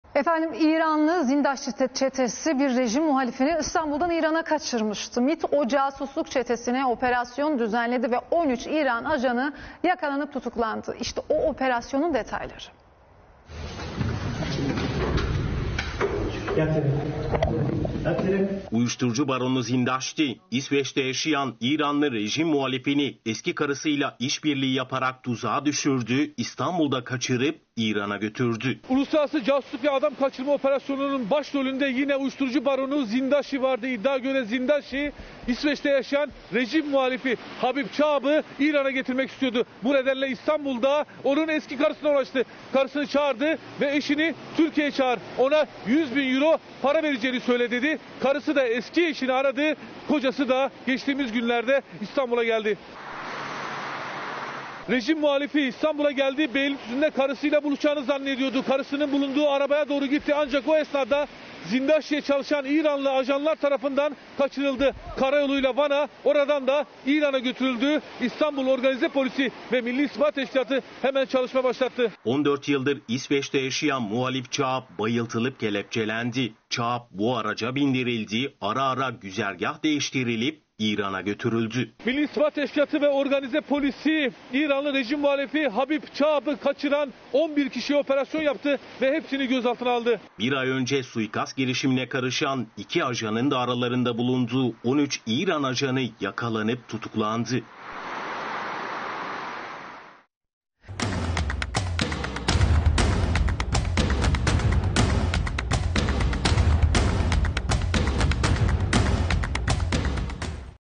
İranlı casusluk çetesine MİT operasyonu!
İRANLI ZİNDAŞTİ ÇETESİ, BİR REJİM MUHALİFİNİ İSTANBUL'DAN İRAN'A KAÇIRMIŞTI... MİT O CASUSLUK ÇETESİNE OPERASYON  DÜZENLEDİ... 13 İRAN AJANI YAKALANIP TUTUKLANDI... İŞTE O OPERASYONUN DETAYLARI...